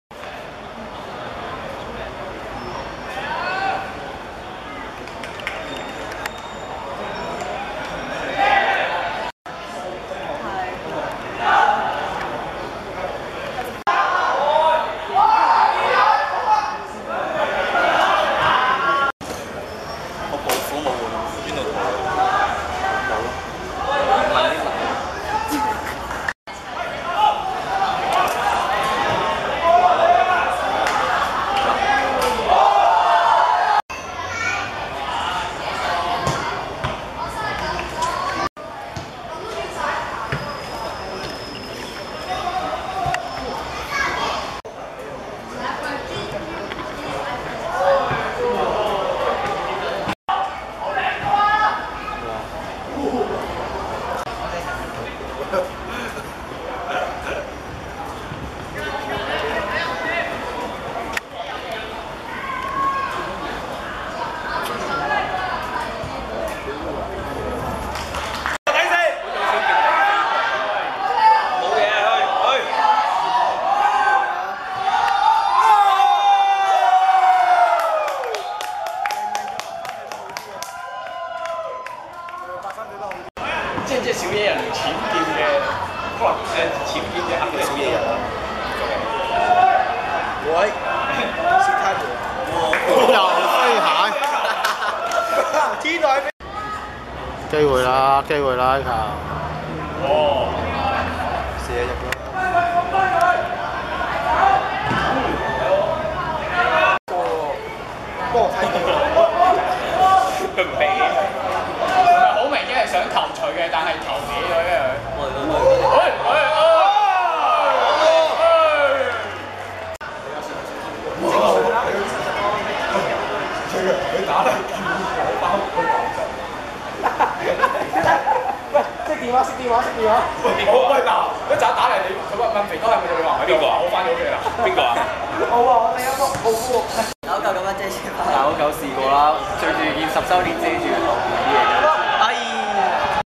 基元vs元朗公立(2012.11.11.元朗學界足球甲組)精華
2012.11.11.元朗大球塲 14:30 上半塲两無記錄 下半塲基元(深藍衫)入一球 賽果 1:0 基元勝出